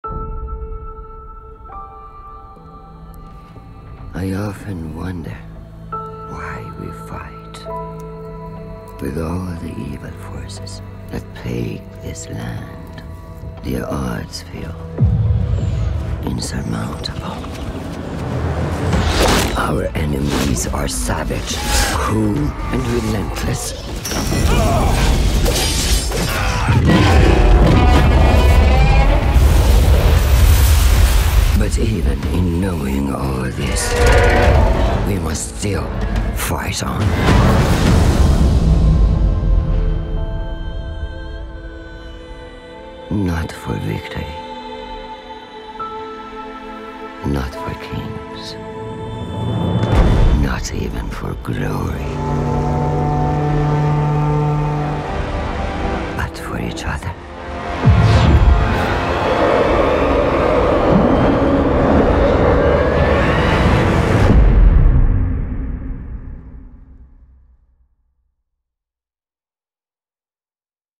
Dragonlance Teaser Trailer | D&D Direct
Narrated by Shohreh Aghdashloo

Dragonlance is coming back to D&D later this year! Battle for the world of Dragonlance across an adventure book and battle game that includes options to link them together as a new way to play D&D.

Dragonlance: Shadow of the Dragon Queen is an adventure book that tells a brand-new story at the beginning stages of the War of the Lance.

Dragonlance: Warriors of Krynn introduces a war game experience to D&D 5th Edition, featuring large scale military battles.